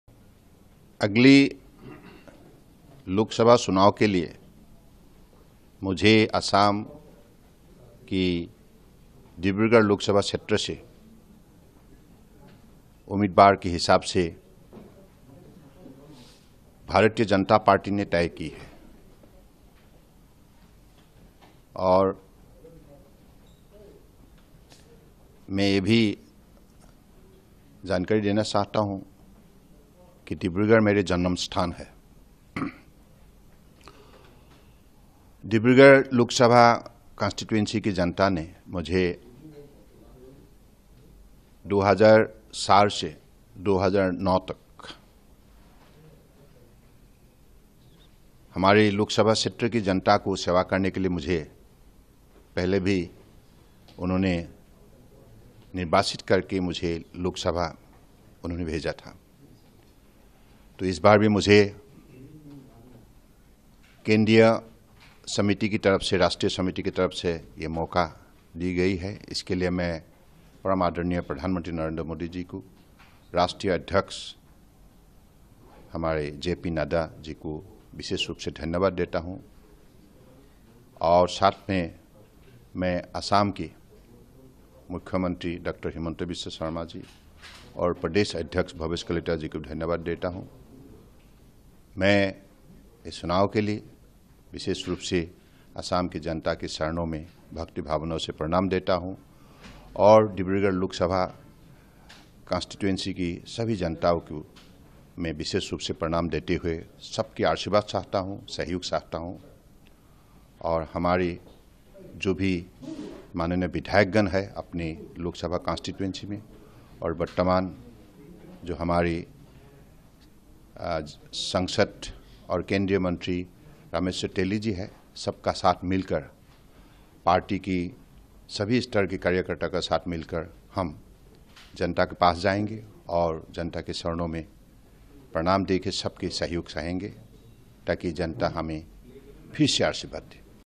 Sarbananda Sonowal News | প্ৰাৰ্থীত্ব লাভৰ পিছত প্ৰতিক্ৰিয়া প্ৰকাশ কৰিলে সৰ্বানন্দ সোণোৱালৰ | N18V
লগতে চাওঁক দিনটোৰ বহু গুৰুত্বপূৰ্ণ খবৰ |